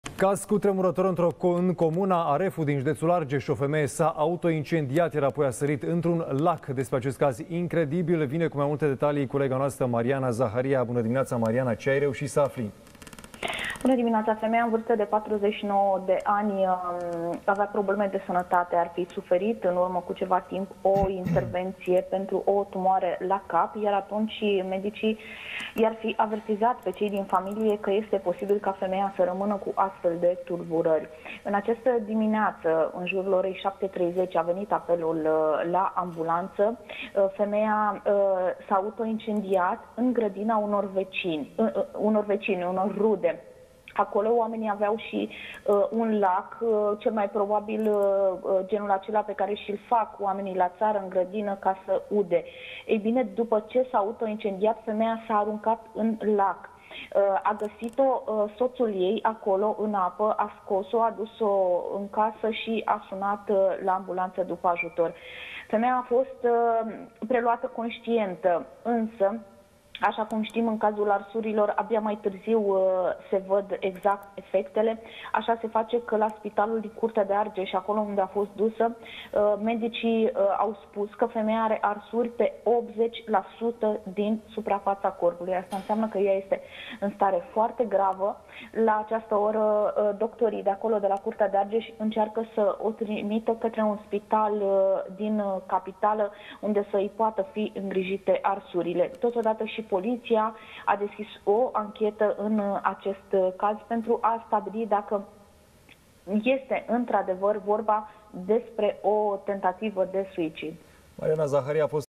Argeș: O femeie și-a dat foc și s-a aruncat în lac
Caz cutremurător în județul Argeș. O femeie și-a dat foc și s-a aruncat în lac, Stirile diminetii, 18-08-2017